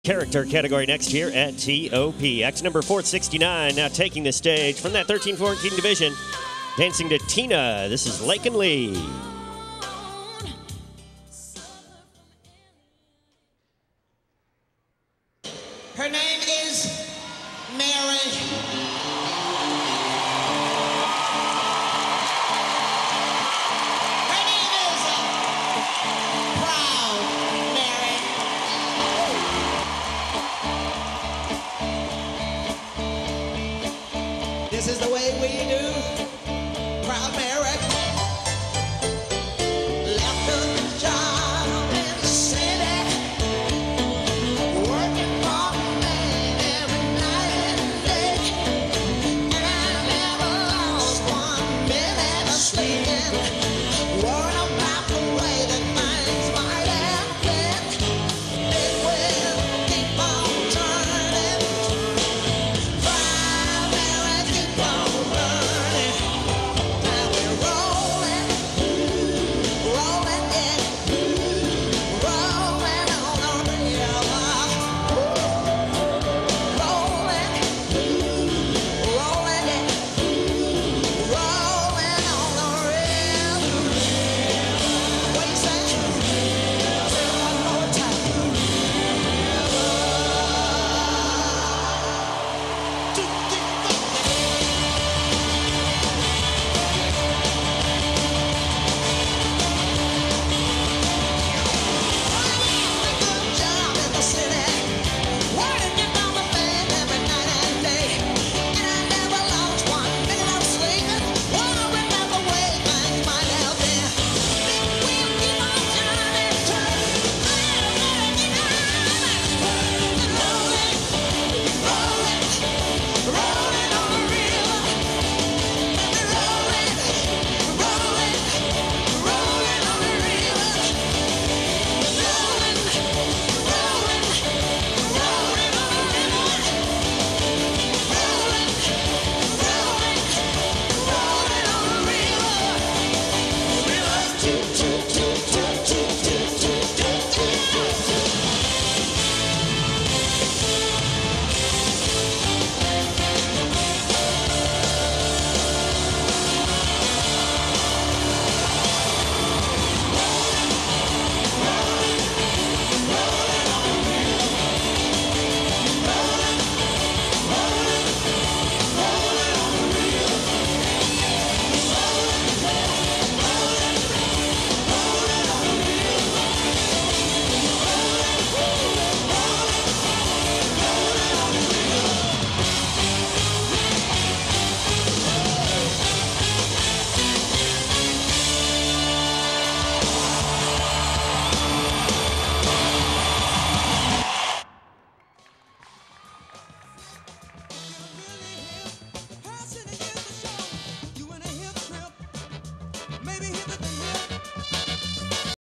TINA! Tina Turner character solo at Talent on Parade performed by Lakyn Lee
TINA! Tina Turner Character solo performed by Lakyn Lee at TOPS Dance Competition in Tulsa, OK on 2/10/13.  She received an Elite Top First!